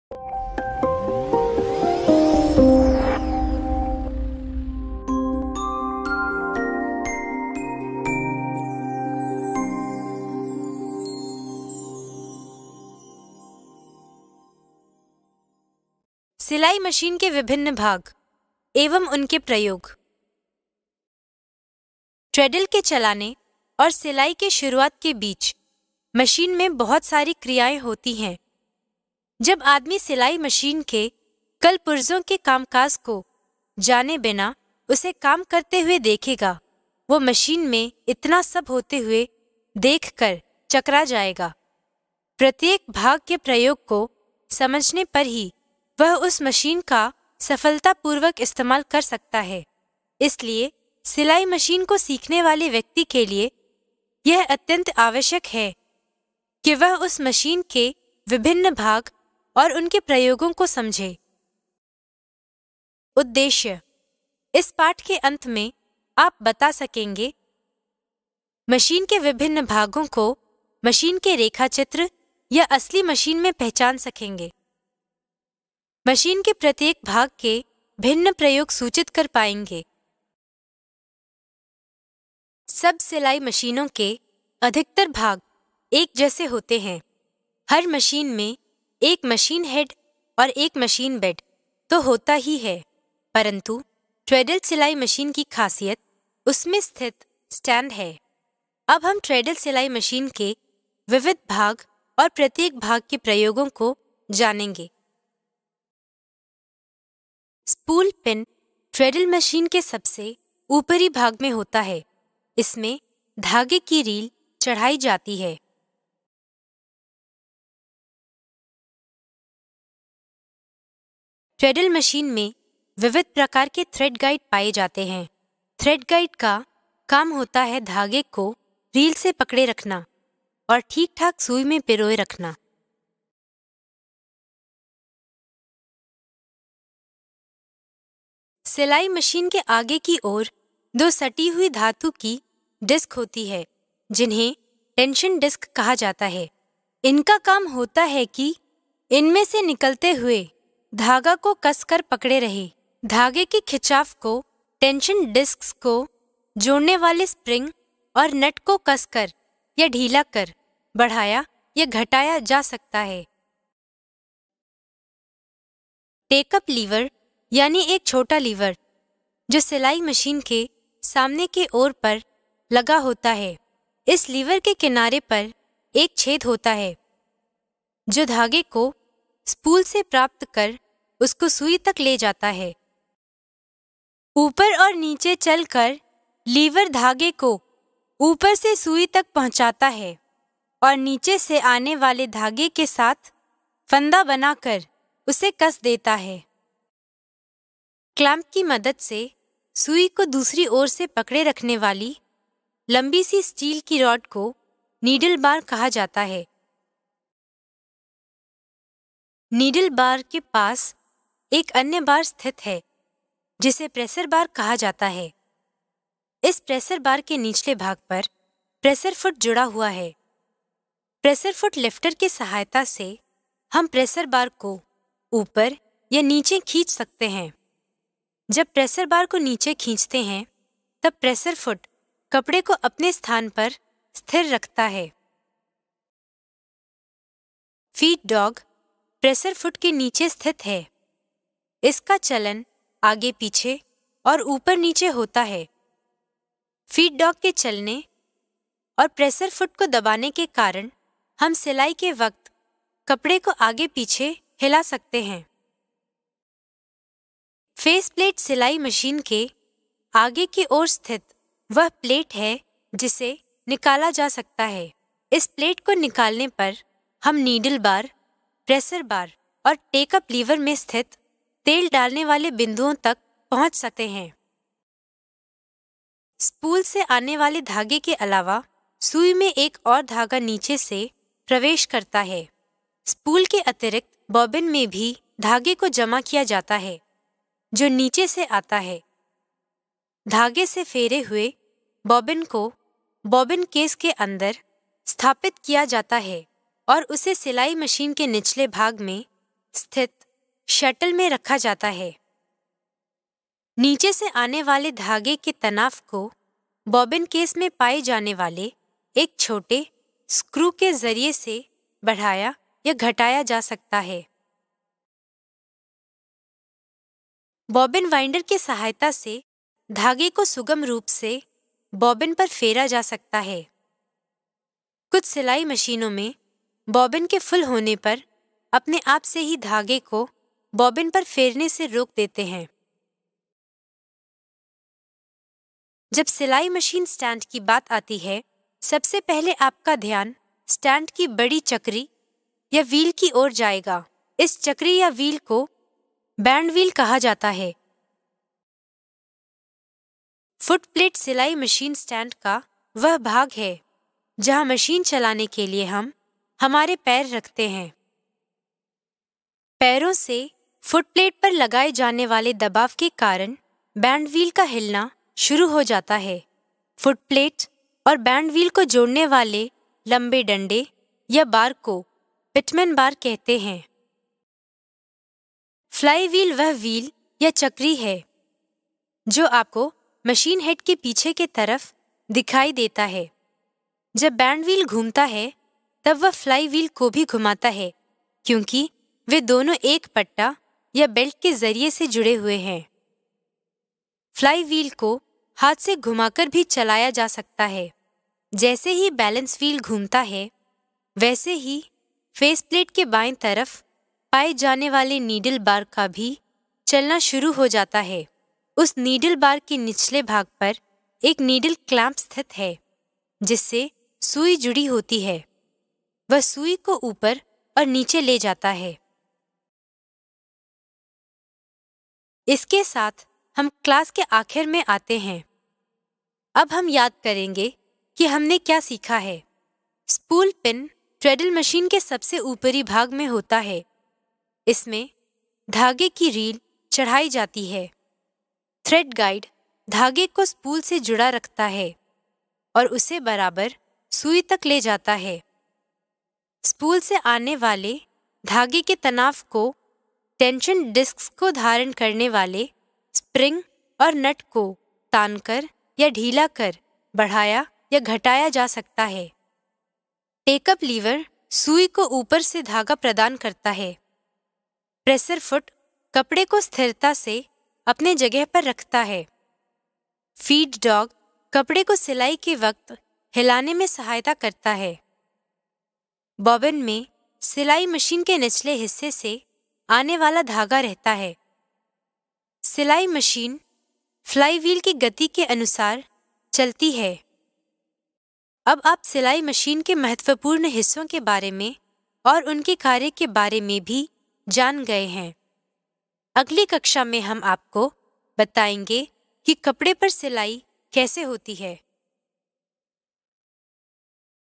1.8.2 Parts And Functions Of Sewing Machine (Hindi) (हिन्दी)
In this video, you will learn about the different parts of a sewing machine and its functions.